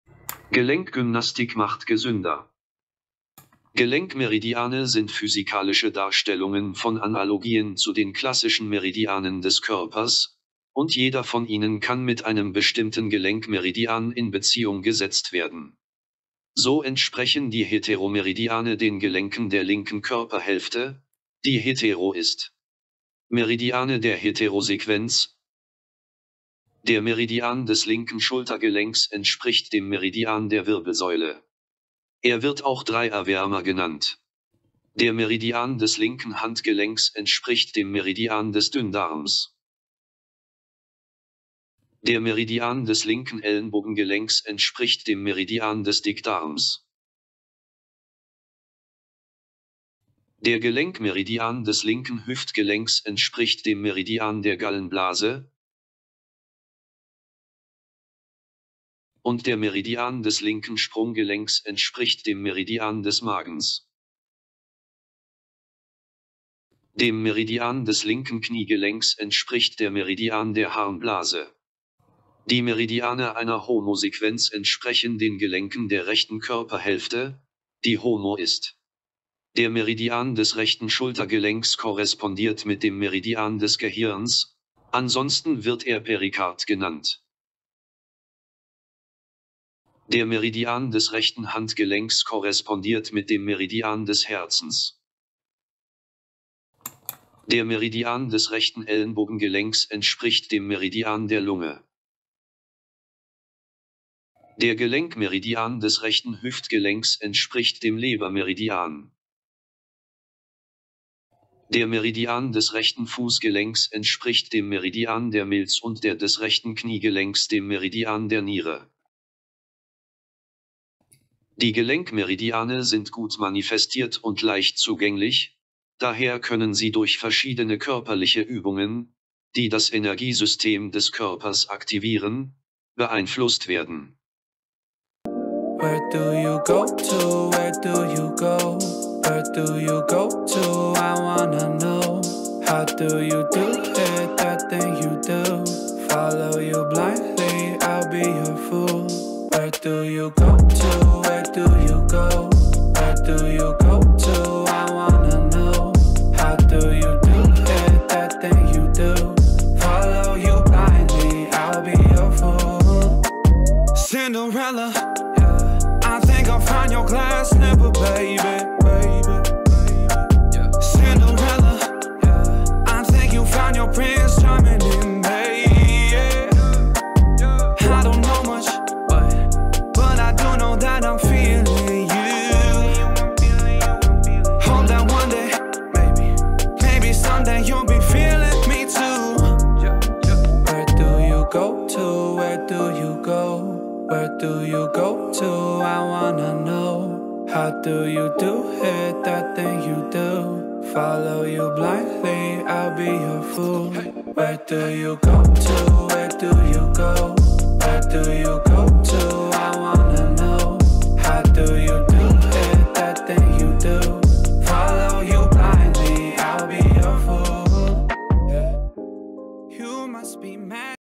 Wie die Gelenke beweglich halten, ist kein Geheimnis mehr! JOINT EXERCISES
JOINT MERIDIANS, JOINT EXERCISES, JOIN TREATMENT
Articular meridians are physical displays of analogs of classical meridians of a body, and each of them can be correlated to the certain articular meridian.                                                                                                                        Thus, hetero meridians correspond with joints of the left half of a body which is hetero.                                                                                                    Meridians of hetero sequence: the meridian of a left shoulder joint corresponds to the meridian of a spinal cord. It is also called a triple heater. The meridian of the left wrist joint corresponds to the meridian of the small intestine. The meridian of the left elbow joint corresponds to the meridian of the large intestine. The articular meridian of the left hip joint corresponds to the meridian of the gall bladder, and the meridian of the left ankle joint corresponds to the meridian of the stomach.                                                                        To meridian of the left knee joint corresponds meridian of a bladder.
Meridians of homo sequence correspond with joints of the right half of a body which is homo.   The meridian of the right shoulder joint corresponds with the meridian of the brain, otherwise it is called pericardium.                                                                                                                 The meridian of the right wrist joint corresponds to the meridian of the heart.
   The meridian of the right elbow joint corresponds to the meridian of the Lung. The meridian of the right hip joint corresponds to the meridian of the liver.                                                                                                                                  The meridian of the right ankle joint corresponds to the meridian of the spleen and of the right knee joint to the meridian of the kidney.  Joint meridians are well manifested and easily accessible, therefore it is possible to influence on them, performing various physical exercises, activating energy system of a body.